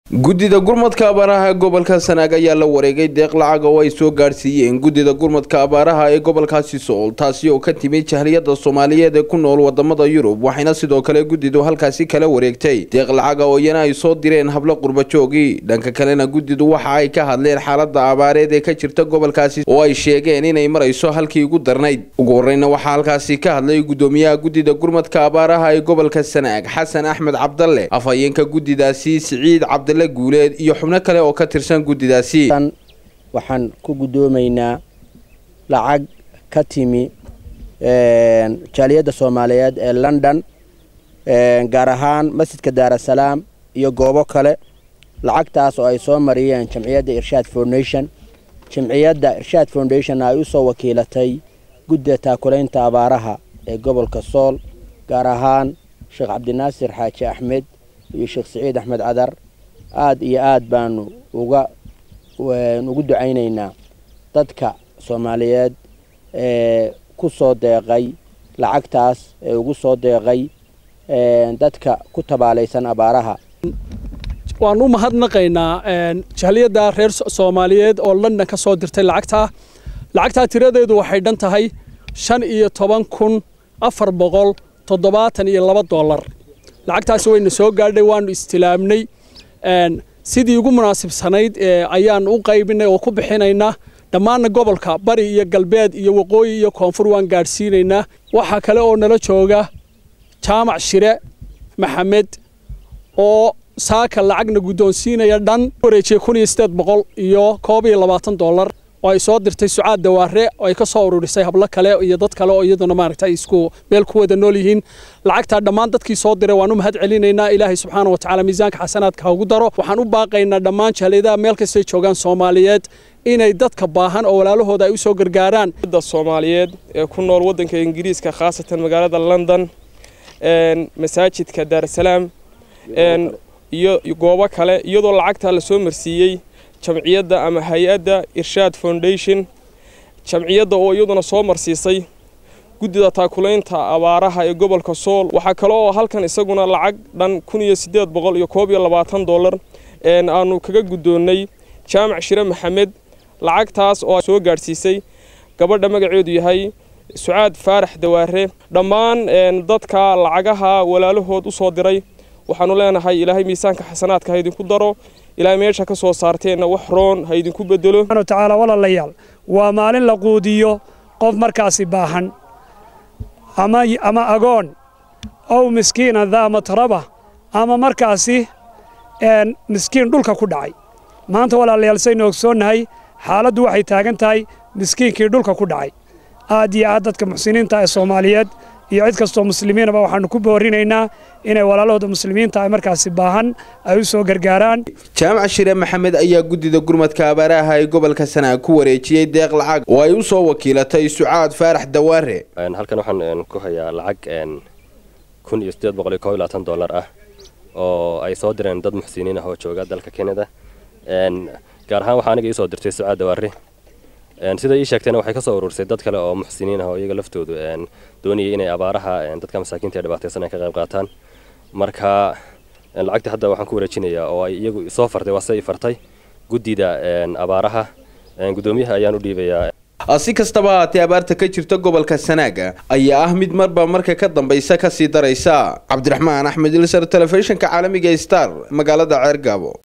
Daawo Gudida Gurmadka Abaaraha G Ssnaag Oo Deeq lacaga Kala Wareegay Gudida Abaaraha G Sool
Reported By Abdirahan Ahmed Elsar